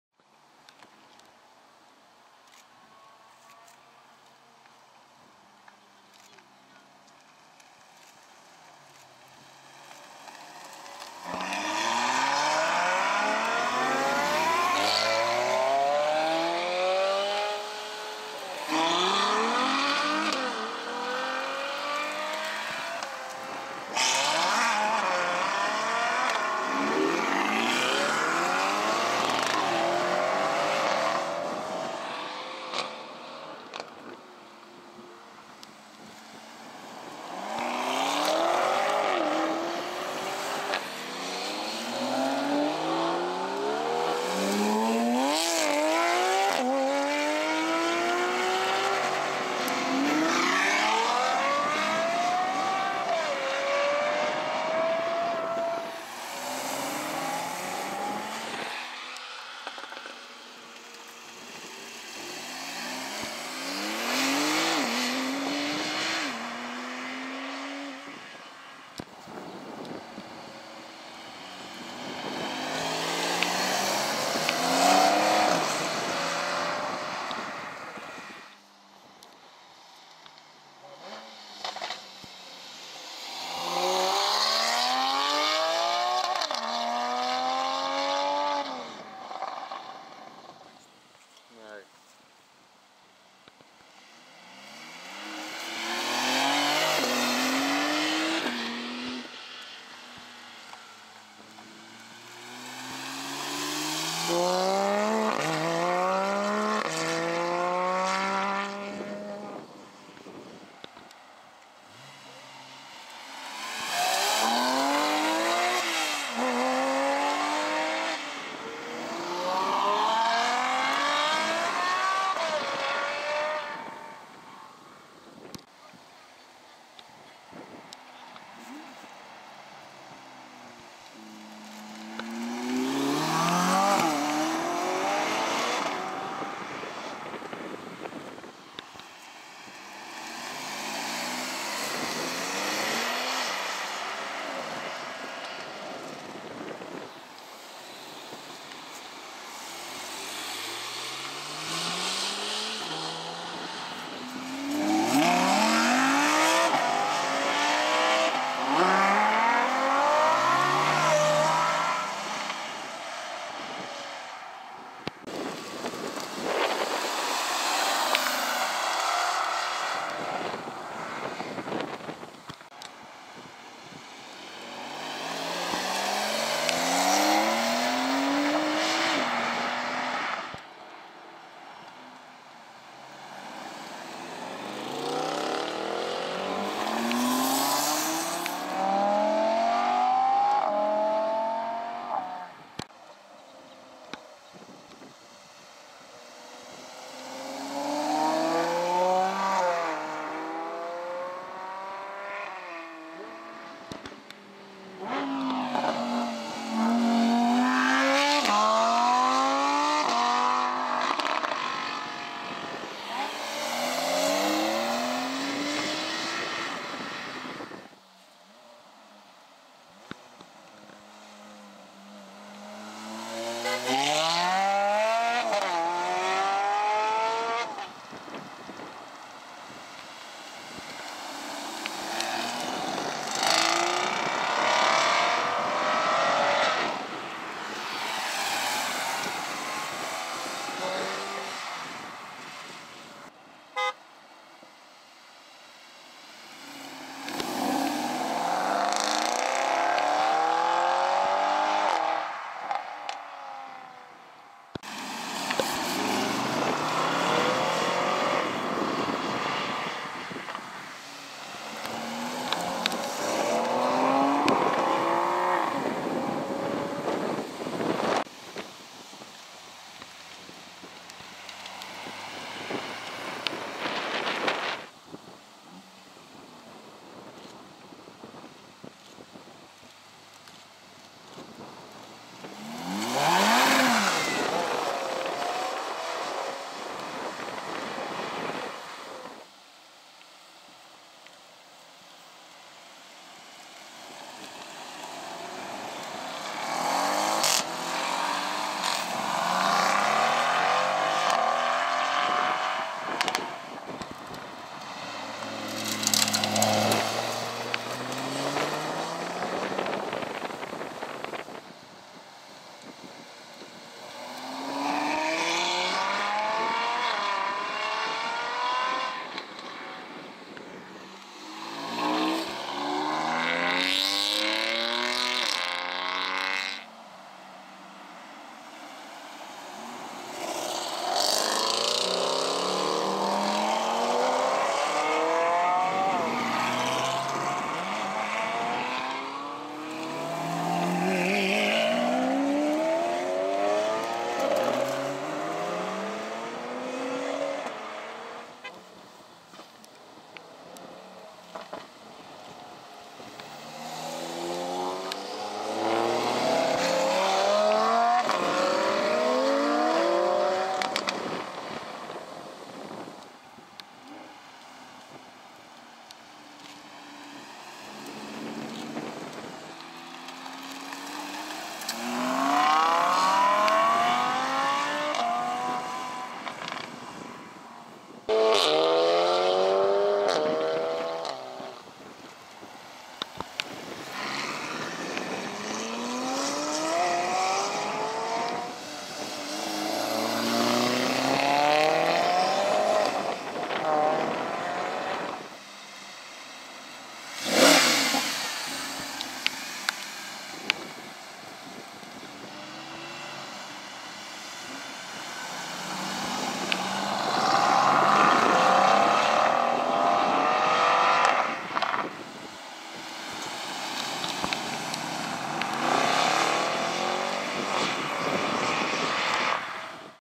Supercars accelerating! Powersliding F8, Honda NSX, SF90, 720s, Huracan Performante and more!
In this video you can see many supercars leaving Cars and Business.

Cars in this video:
- Ferrari F430
- Ferrari GTC4 Lusso
- Ferrari 812 Superfast
- Ferrari Portofino M
- Mercedes C63s AMG
- Lamborghini Urus
- Porsche 911 992 Carrera
- Aston Martin DBS Superleggera
- Porsche GT3RS with IPE exhaust
- Mercedes AMG GTR PRO
- Ferrari GTC4 Lusso with Novitec exhaust
- Porsche 911 991 Carrera
- Porsche 911 992 Turbo S
- Porsche 911 992 Carrera Techart
- Porsche 911 991 Targa
- Porsche GT3RS
- Porsche GT3 Touring
- Ferrari GTC4 Lusso
- Ferrari SF90 Stradale
- Audi RS6 C8
- Porsche 911 991 Turbo S
- Bentley Continental GTC
- New Audi R8
- Ferrari 812 Superfast
- Lamborghini Huracan Performante spyder
- Ferrari F430 spider
- Aston Martin Virage
- Porsche GT3 Touring
- Mercedes SLS AMG
- BMW M850i
- Aston Martin DB11
- Mclaren 720s MSO
- Ferrari F8 Tributo
- Audi RS6 C7
- Mercedes AMG GTC
- Donkervoort D8 GTO JD70
- Ferrari 812 Superfast
- BMW M4
- Range Rover SVR
- Ferrari 458 spider
- Ferrari 458
- Lamborghini Aventador
- Honda NSX
- Audi RS4
- Lamborghini Huracan spyder
- Mclaren 720s spider
- Audi R8 V10 +
- Porsche GT3RS
- Mclaren 600LT spider
- Donkervoort D8 GT Coupe
- Mercedes SLS AMG
- Mercedes AMG GTS
- BMW M850i